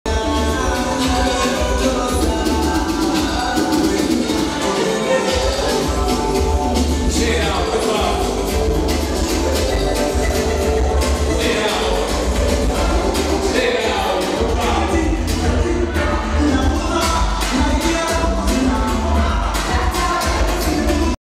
Girls Day Out !! Girls having fan. Ladies Night
Girls enjoying a Saturday Ladies night.